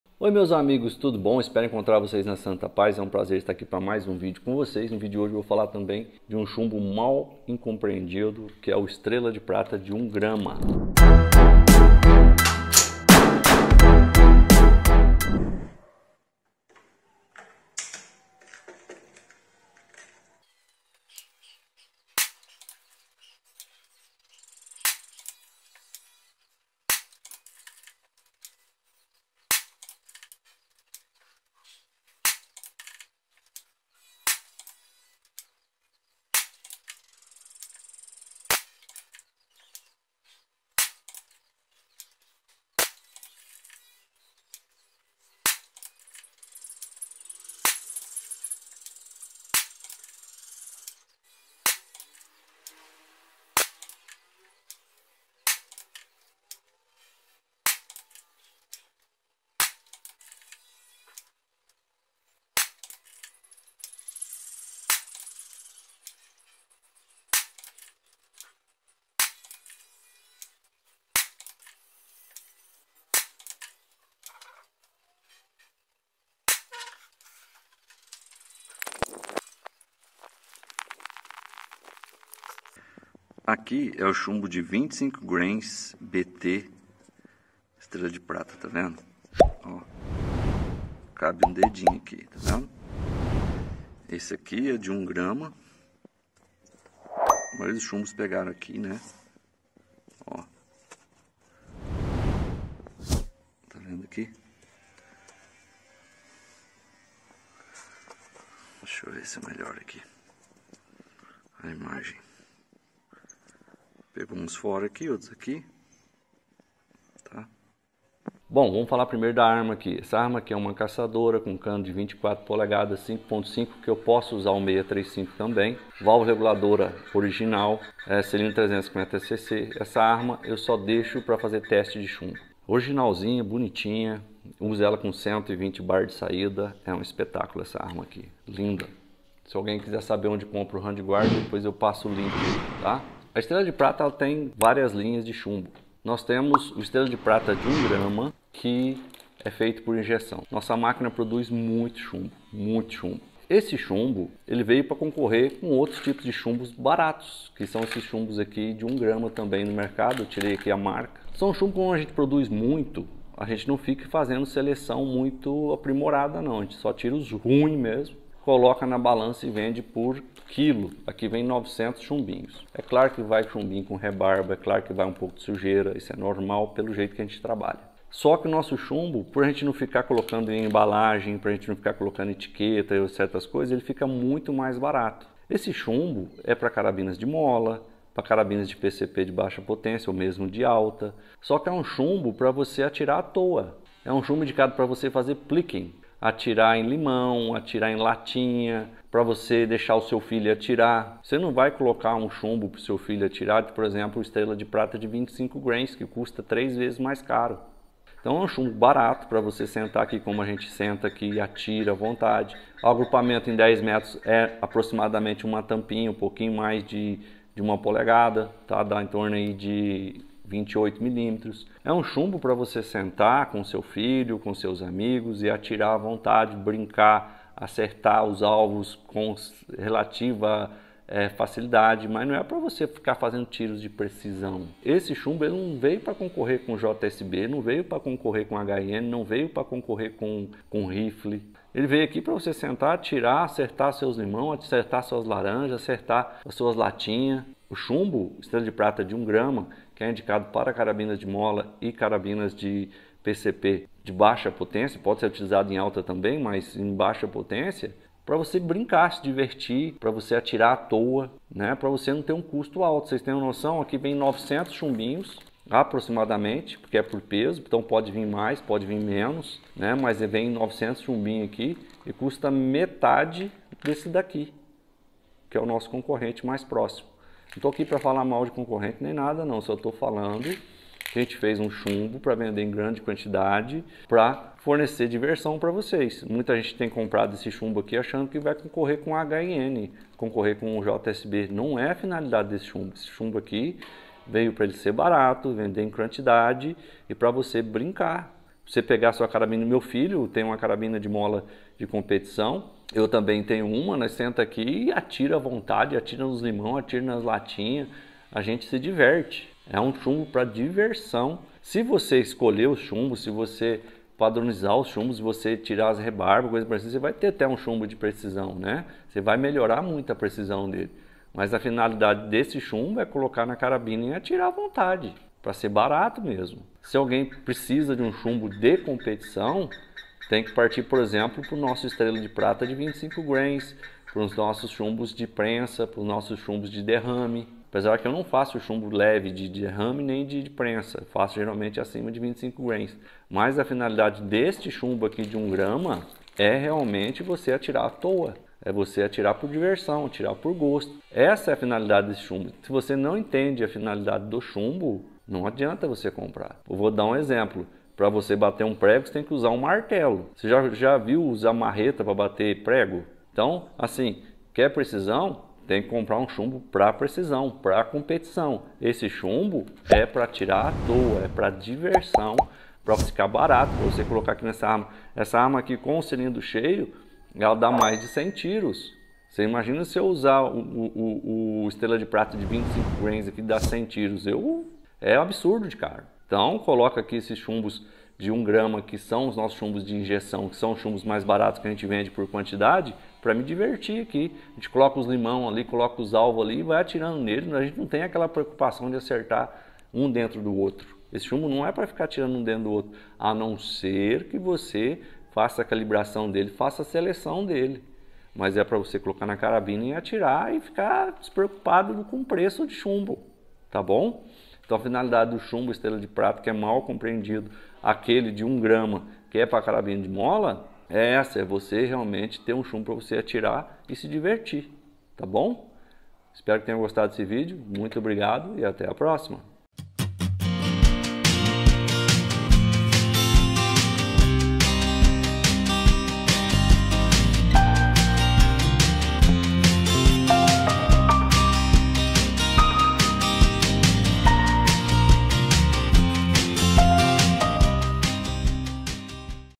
Diabolô Estrela de Prata 5,5mm 1 grama - O Chumbinho mal feito?
Os chumbinhos slugs para airguns são projetados para melhorar a precisão e poder de impacto dos chumbinhos tradicionais. Eles têm formato aerodinâmico semelhante a uma bala de arma de fogo e podem ser feitos de chumbo, latão ou cobre. Os preços variam de acordo com o fabricante, material, calibre e peso. É importante testar diferentes tipos de chumbinhos para encontrar o mais adequado para sua airgun e atividade desejada. Os slugs surgiram nos Estados Unidos e são usados em competições de tiro, caça e tiro ao alvo em longas distâncias.

O vídeo em questão é sobre um chumbo de ar comprimido chamado "Top Ogival Ponta Oca 25 grains", fabricado pela empresa "Chumbo Estrela de Prata". O apresentador fala sobre as características da arma utilizada nos testes, uma carabina de cano 5.5mm de precisão, e menciona que a empresa produz três modelos de chumbos, sendo dois deles abaulados. Ele também fala sobre a empresa em si, que é uma das duas empresas pertencentes aos apresentadores e que o chumbo é produzido com 99% de pureza. O apresentador ainda comenta que o chumbo não é destinado a competir com outros chumbos de alta qualidade, mas é adequado para atividades de lazer e testes de precisão. O vídeo não contém informações relevantes sobre o uso ou manuseio de armas de ar comprimido.
O vídeo aborda o chumbinho de 1 grama "Estrela de Prata", que é fabricado por injeção e produzido em grande quantidade. É um chumbo barato e destinado para diversão, recomendado para carabinas de mola e carabinas de PCP de baixa potência, podendo ser utilizado em alta potência também. O objetivo desse chumbo é ser vendido em grande quantidade e para fornecer diversão, não para competição ou precisão. O chumbo é vendido por peso e pode conter algumas impurezas, como rebarbas, mas pode ser padronizado para melhorar sua precisão. O vídeo também mostra a carabina utilizada para testar o chumbinho e a forma como é comercializado.